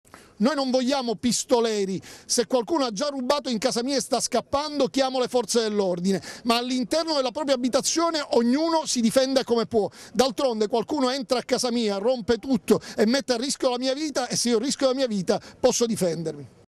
Legittima Difesa, IDV: non vogliamo pistoleri ma in casa propria ognuno si difenda come può
(Agenzia VISTA) - Roma, 1 Giugno 2016 - Oltre un milione di firme certificate raccolte a sostegno della proposta di legge di iniziativa popolare per rinforzare la legittima difesa. Le ha presentate
l'Italia dei Valori nel corso di una iniziativa in piazza Montecitorio. I militanti del partito hanno marciato dalla loro sede, Via Santa Maria in Via, fino al Senato, dove le firme sono state depositate.